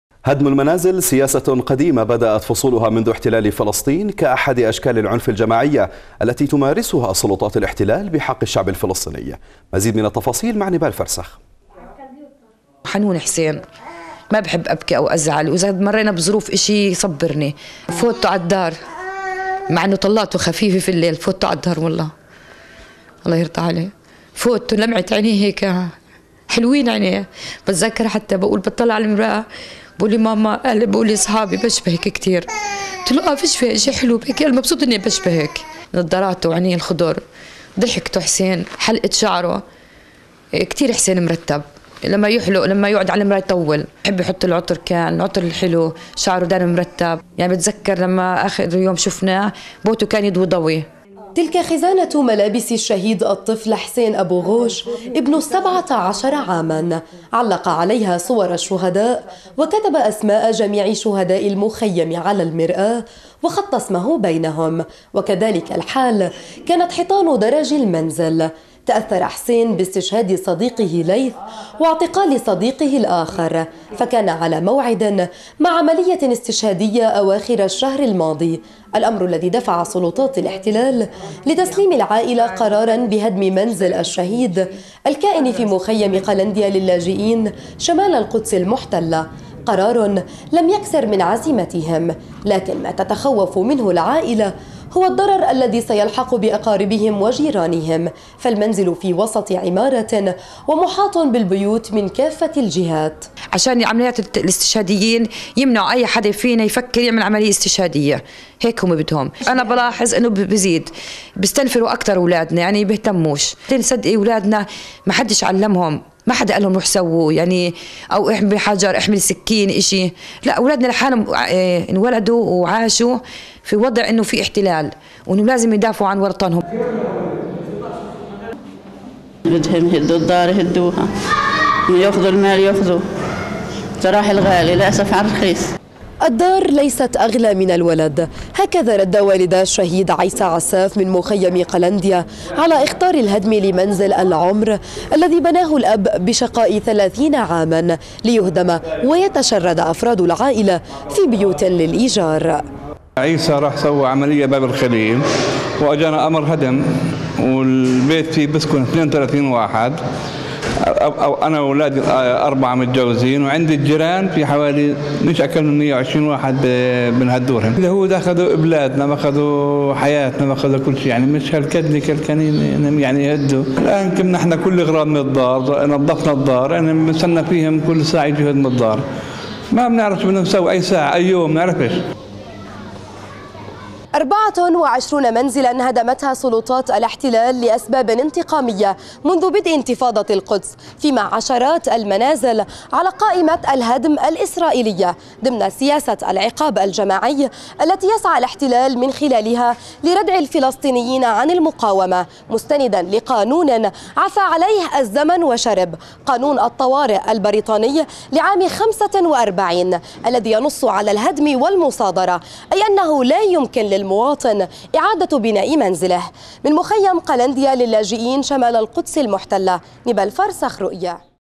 سلطات الاحتلال الإسرائيلي تواصل سياسة هدم منازل الفلسطينيين - (25-2-2016)
هدمُ المنازلِ ... سياسةٌ قديمةٌ بدأت فصولُها منذ  احتلالِ فلسطين، كأحدِ أشكالِ العنفِ الجماعية التي تمارسها سلطاتُ الاحتلالِ بحق الشعب الفلسطيني.

تقرير: نبال فرسخ.